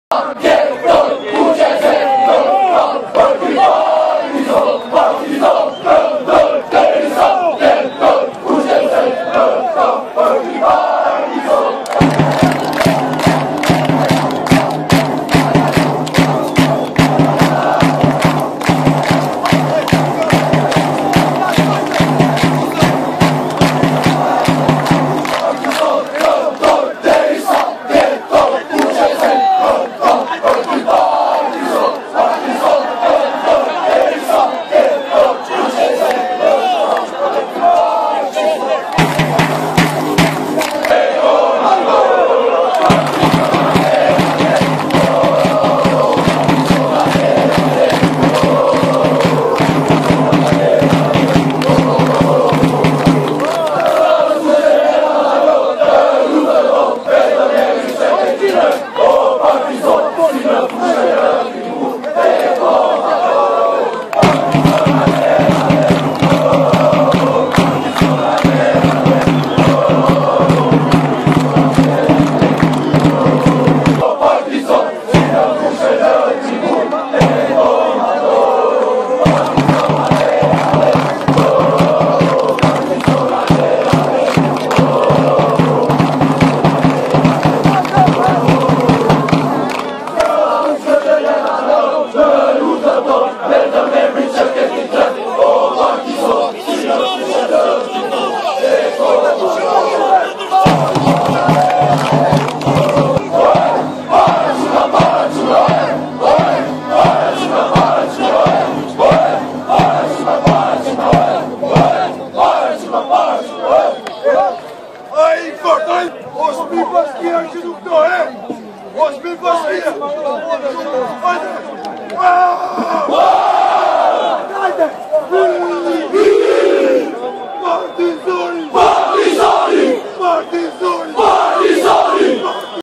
Ultras Guerrils. Kukesi 3 - 0 Partizoni
Guerrils nuk pushuan per asnji sekond dhe pse ndeshja ishte 3-0  Per kuksin